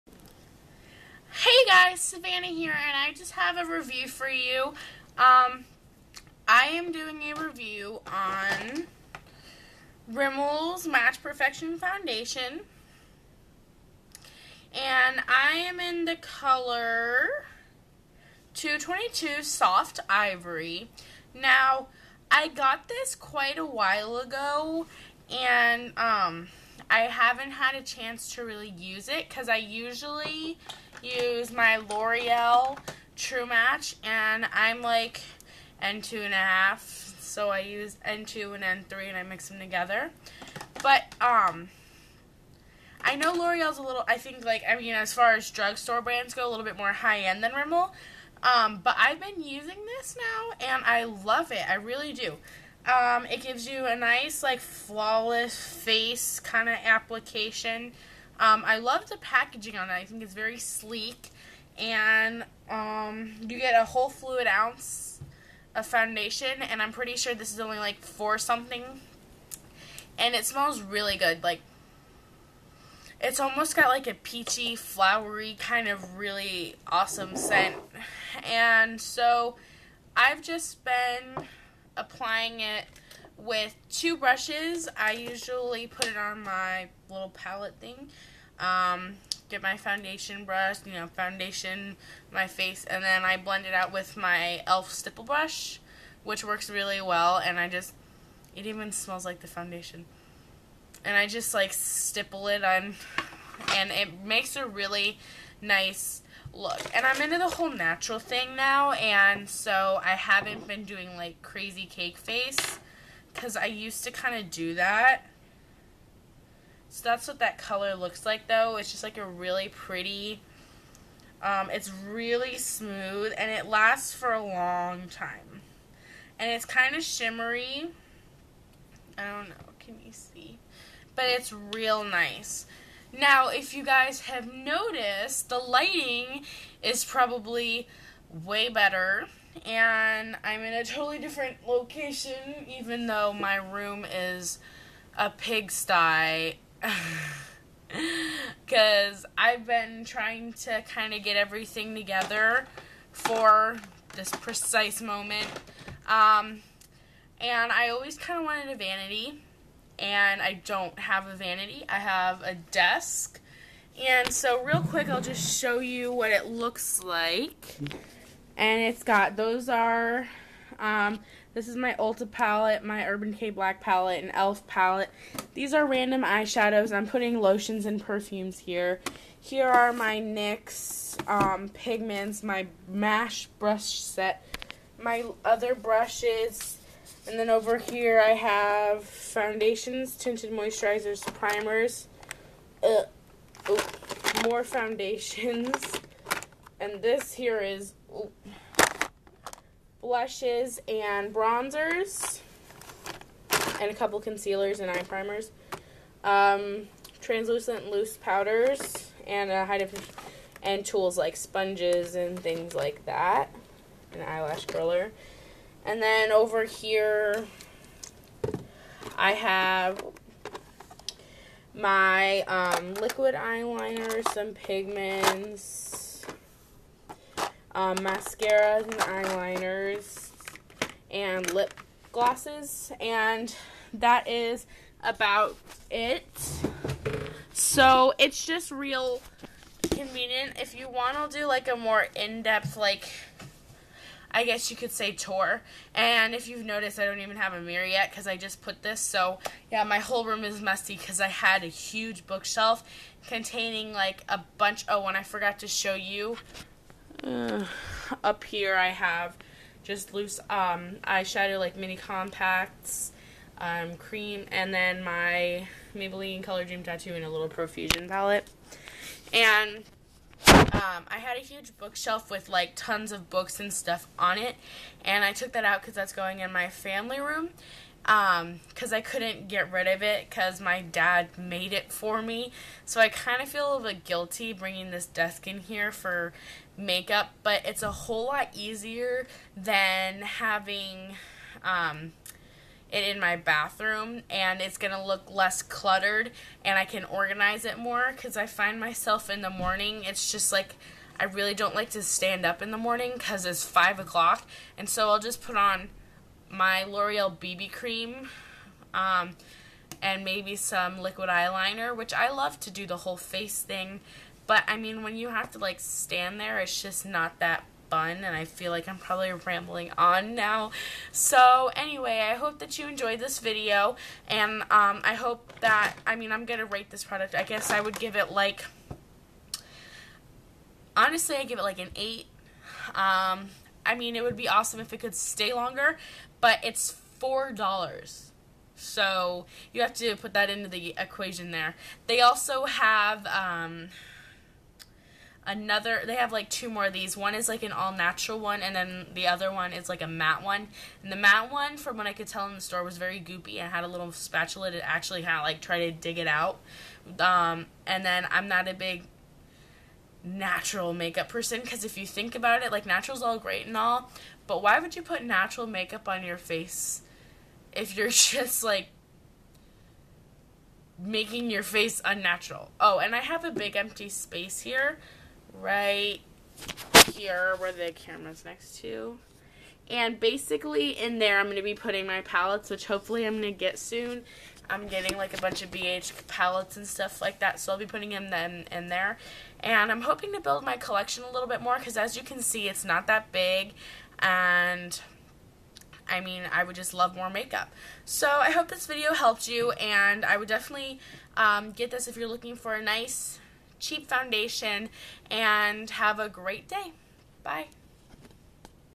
Review: Rimmel Match Perfection Foundation
I am in the color 202 soft Ivory
This is a great deal for a little over four dollars pick it up at your local drugstore today!